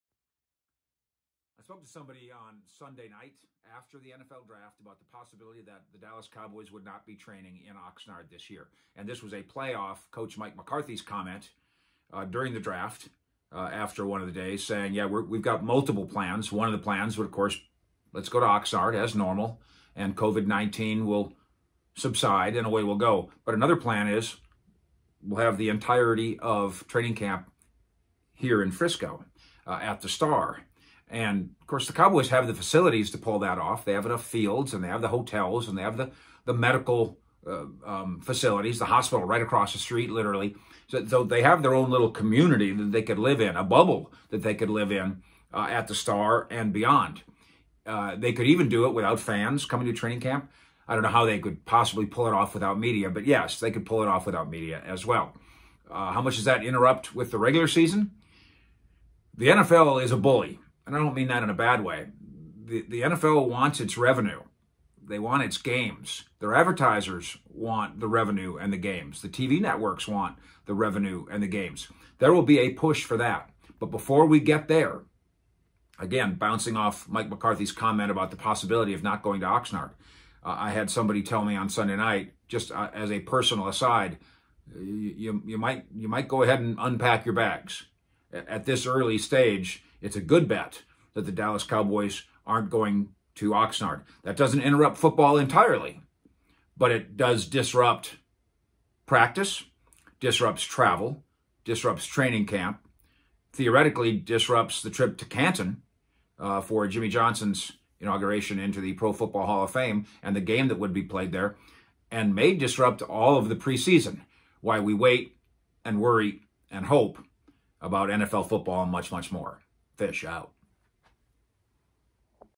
FISH REPORT: Could Cowboys Cancel Camp in Oxnard?
FISH REPORT: Could the Dallas Cowboys Cancel Training Camp in Oxnard?